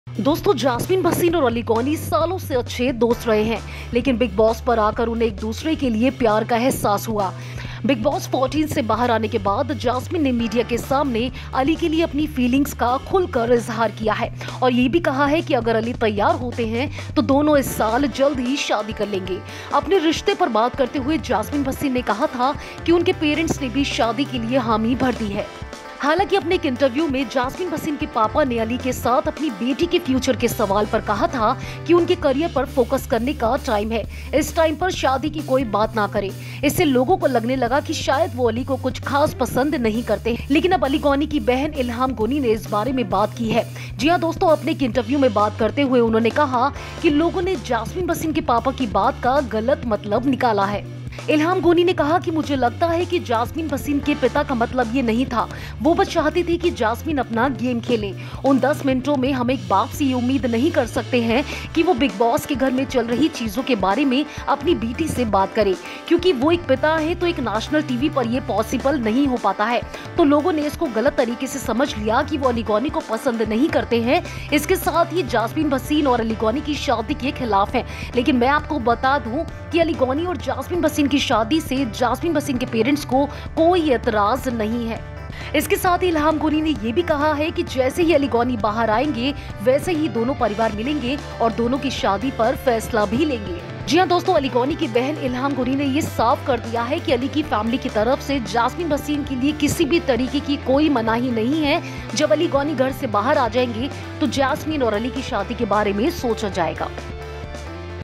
Bigg Boss 14: Aly Goni की बहन ने किया Jasmin के पिता के Reaction का बचाव | FilmiBeat
Bigg Boss 14: Jasmin Bhasin’s parents are fine about her relationship with Aly, confirm sister Ilham Goni.Watch Video To Know More